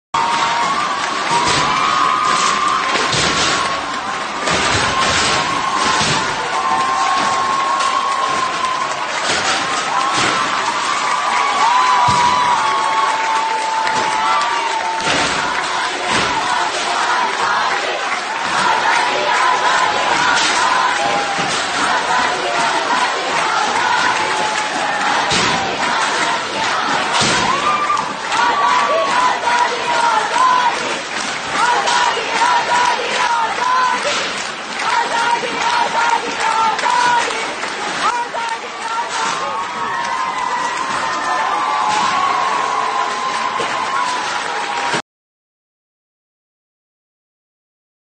Un grupo de universitarios derriba el muro de la cafetería que los separa de las mujeres en Irán
En esta localidad costera del sur del país, un grupo de estudiantes hombres decidió derribar el muro de la cafetería de la universidad que separa el sector masculino del femenino.